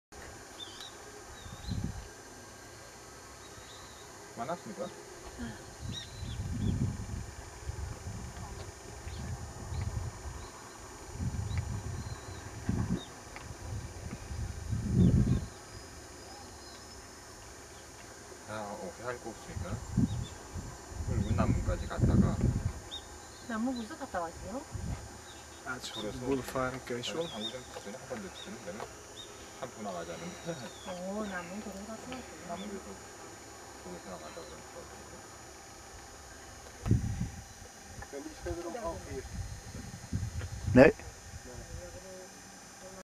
Kaesong
View on Kaesong, the North Korean border town close to the DMZ. The video was shot from a hill with a giant Kim Il Sung statue. A police officer can be seen aranging traffic down the road.